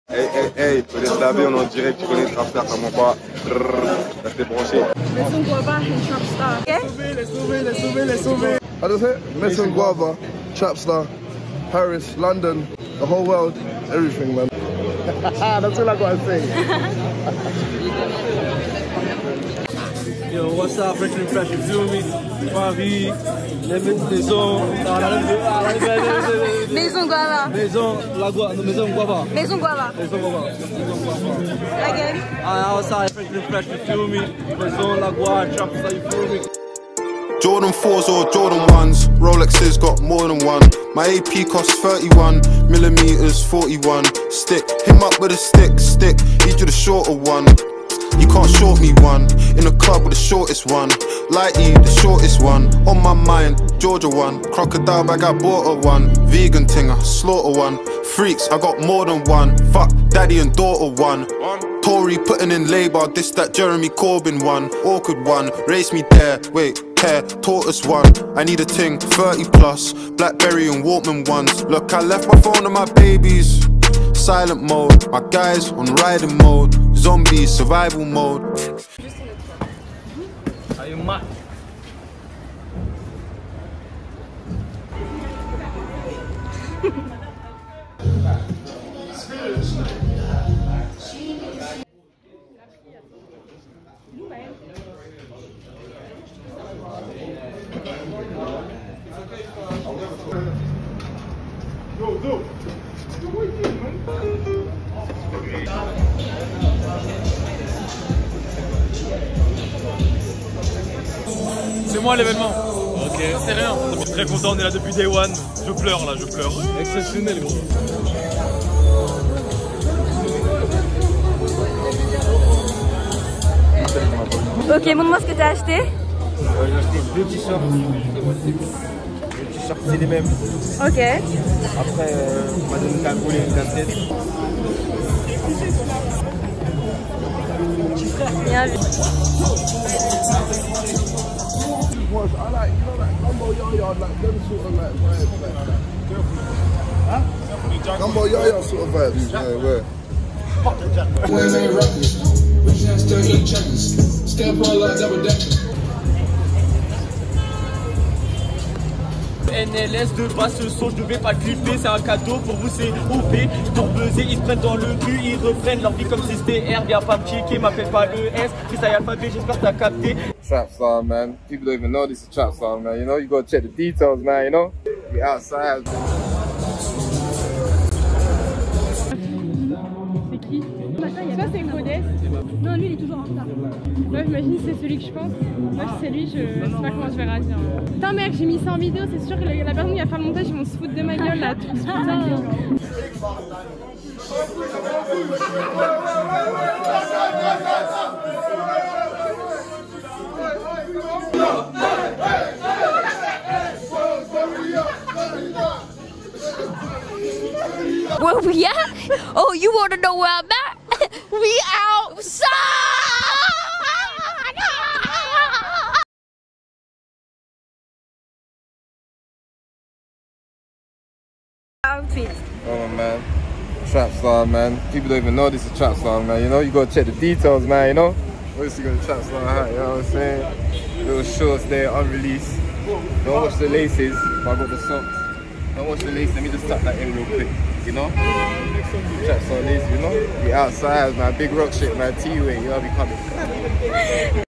TRAPSTAR IN PARIS
On a eu l'honneur de recevoir la marque Trapstar au 48 rue Cambon pendant la Paris Fashion Week.